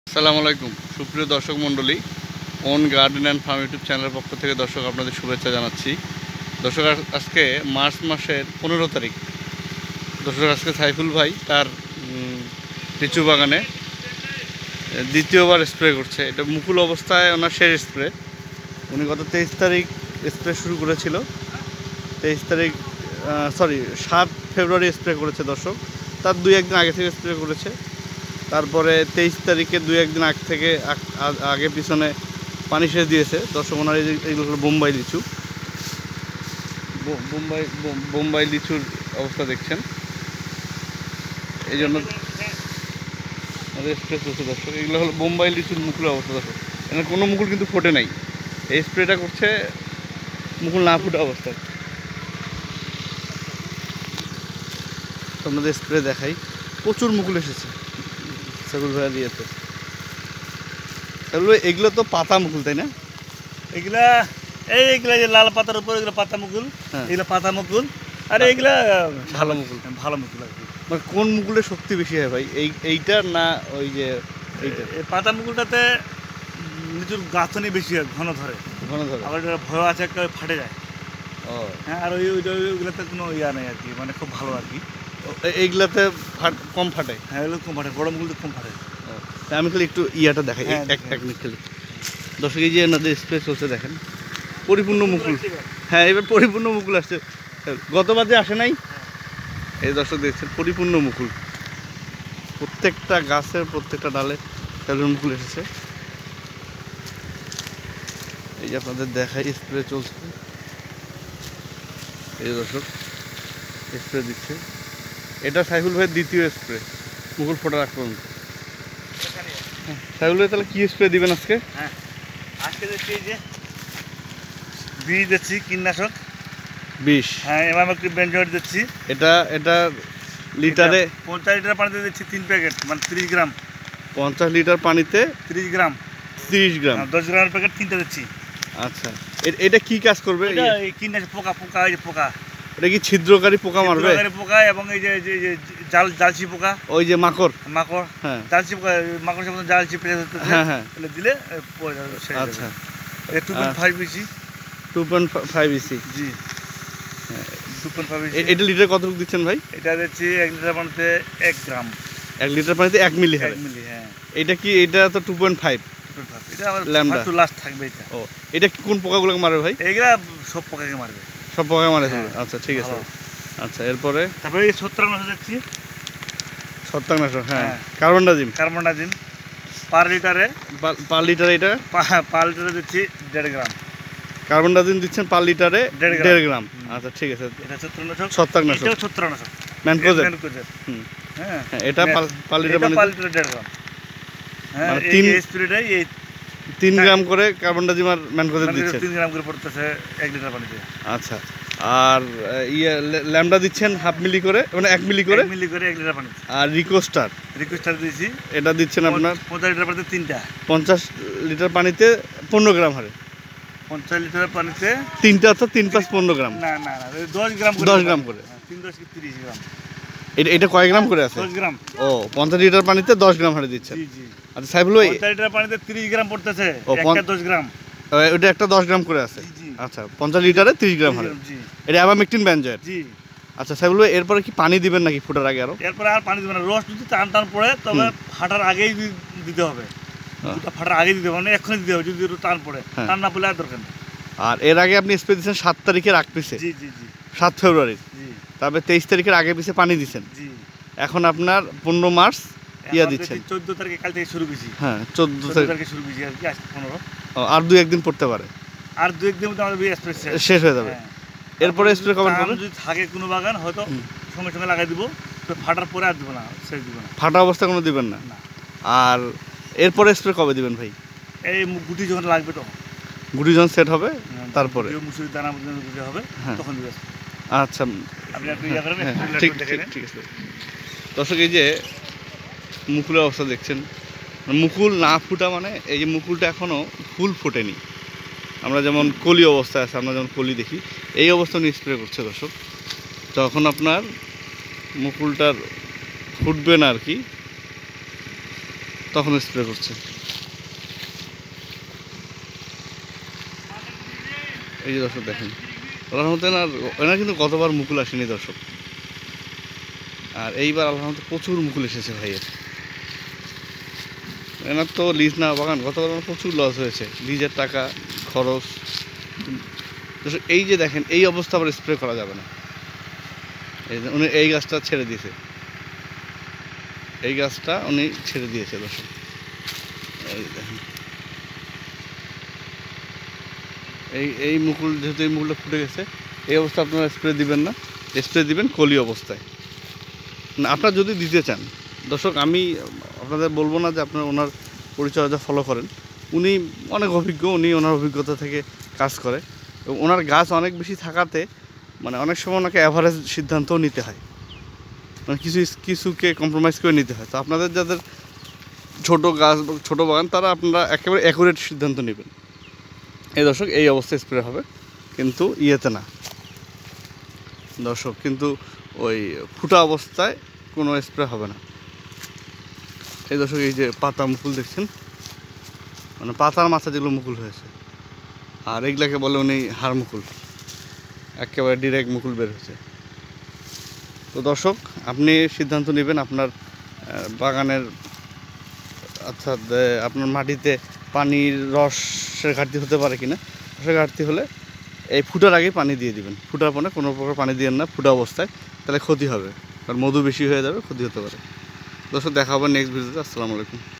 লিচুর মুকুল ফোটার আগে শেষ স্প্রে ও প্রয়োজনে পানি সেচ ( সাইফুল ভাই -০৩) ১৫/৩/২০২৪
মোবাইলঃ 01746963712
মোঃ মিজানুর রহমান